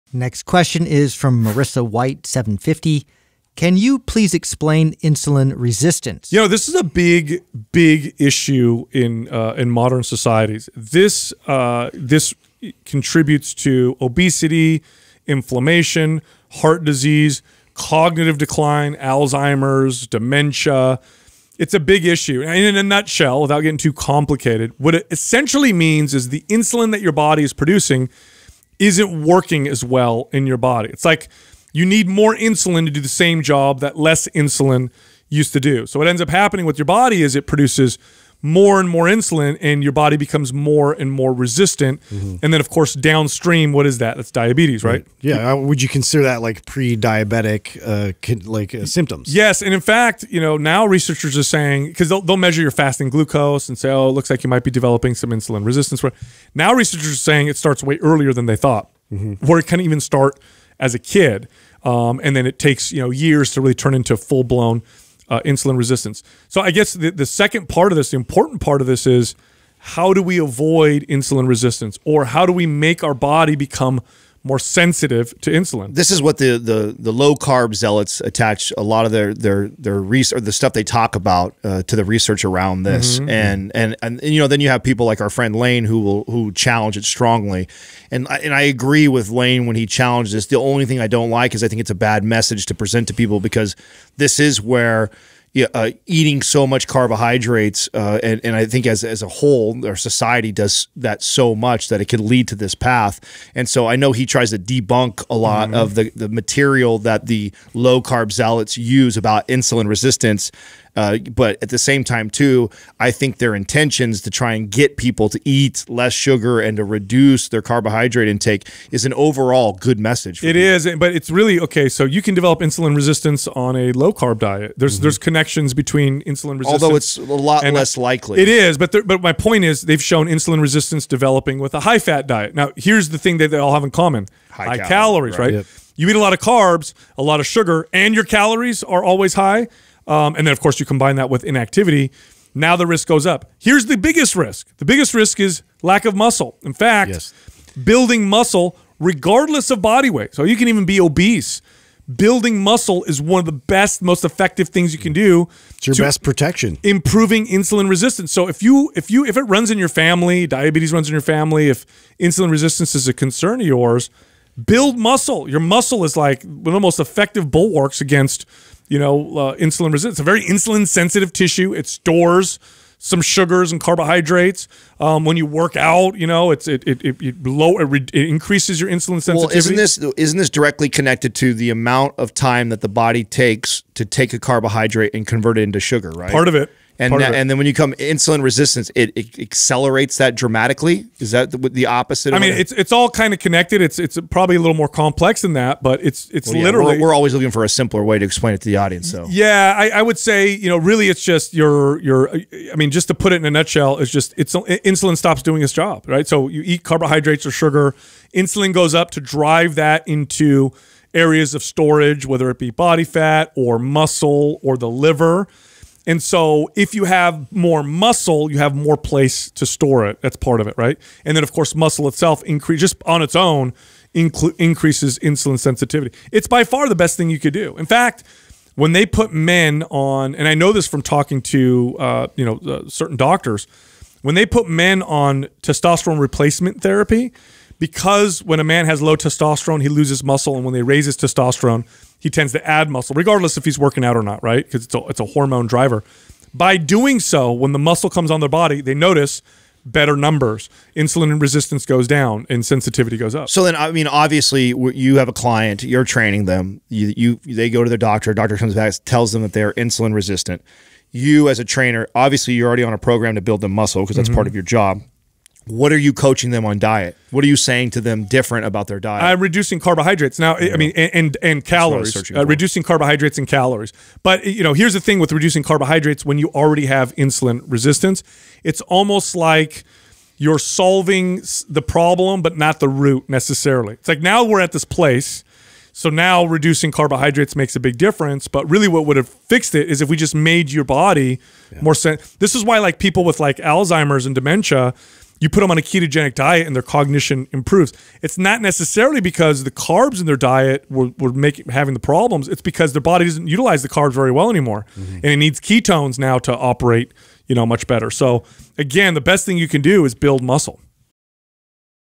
What Is Insulin Resistance & How To Prevent It
In this QUAH Sal, Adam, & Justin answer the question “Can you please explain insulin resistance?"